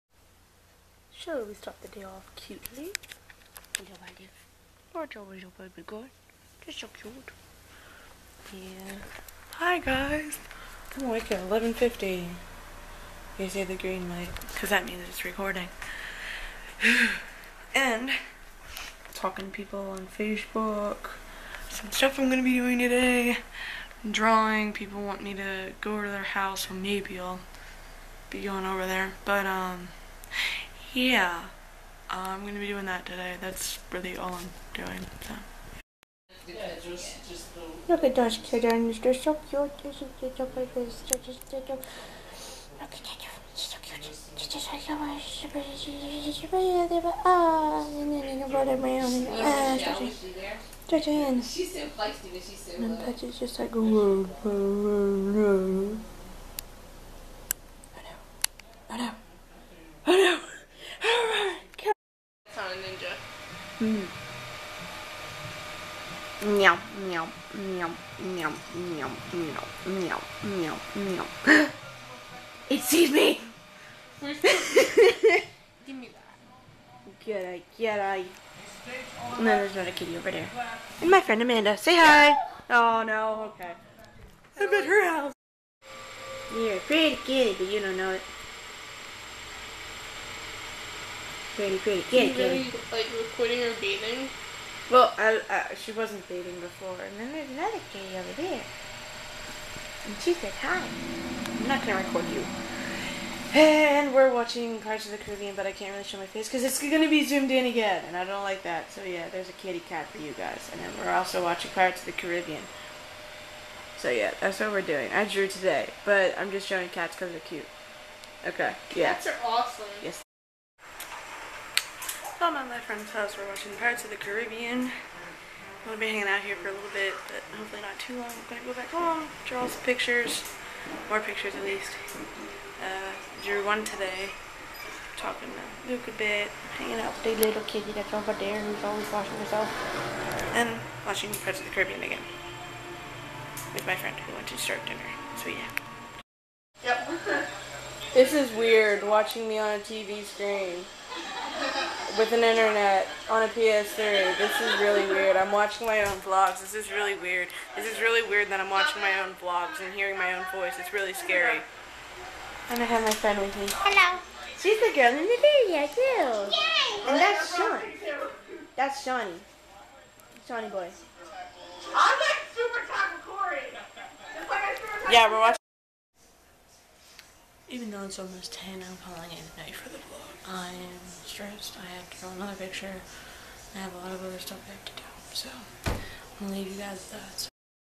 KITTIES EVERYWHERE (Vlog #6, 6.12.12)
Went over to a friend's house for a bit and drew.  That's what happened.  Sorry I didn't have anything vlogged for the other two days.  Never really got to vlog.